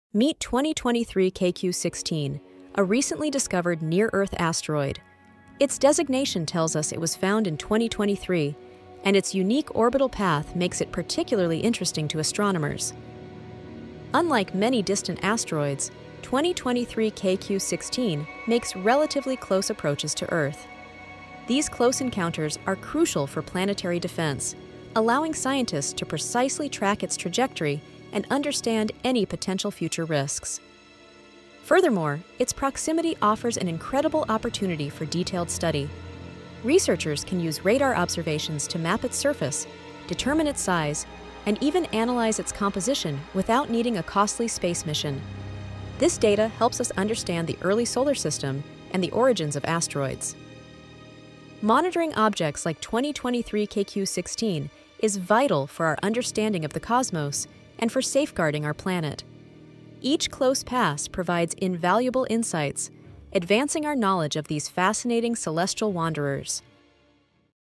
An asteroid near Earth? What is 2023 KQ16?
As of now, there is no new information about asteroid 2023 KQ16 beyond its original discovery. 2023 KQ16 is a relatively small asteroid discovered on May 27, 2023.

Here are the most important known facts:

Size: Its diameter is estimated to be approximately 11 to 25 meters, making it one of the numerous small celestial bodies in our solar system.
Orbit: It belongs to the category of Near-Earth Objects (NEOs), whose orbit crosses that of Earth.
Safe Pass: At the time of its discovery, it was already on its way away from Earth. It passed our planet at a very large and safe distance of over 2.2 million kilometers.
The discovery of such asteroids is a routine process for astronomical sky survey projects like the Catalina Sky Survey. They help us better understand the population of smaller objects in our cosmic neighborhood. There are no indications that 2023 KQ16 could pose a threat to Earth in the foreseeable future.